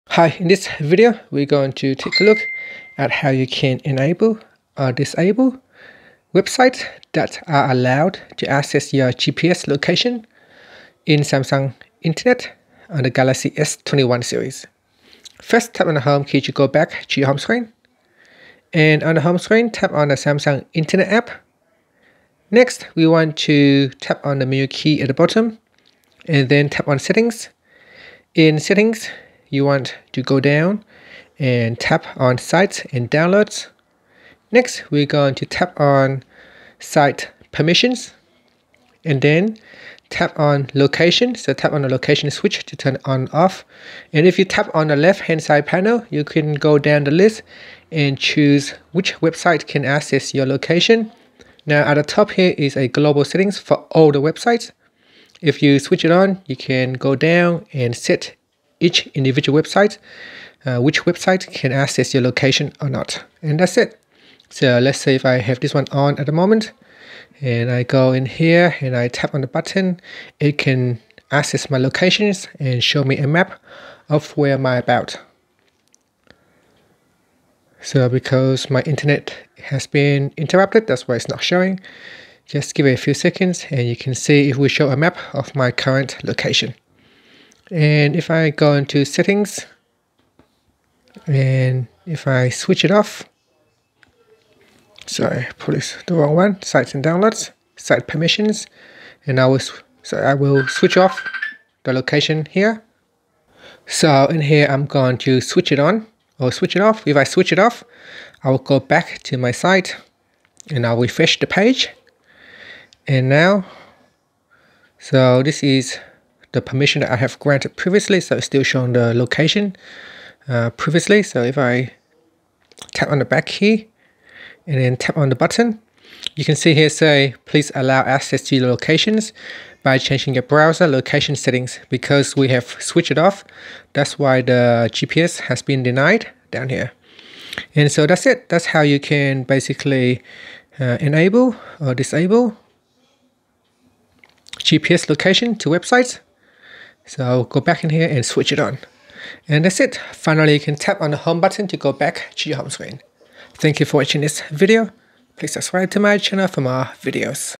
Galaxy S21/Ultra/Plus: How to Enable/Disable Websites That Are Allowed to Access Your GPS Location
Learn how you can enable or disable websites that are allowed to access your GPS location on the Galaxy S21/Ultra/Plus.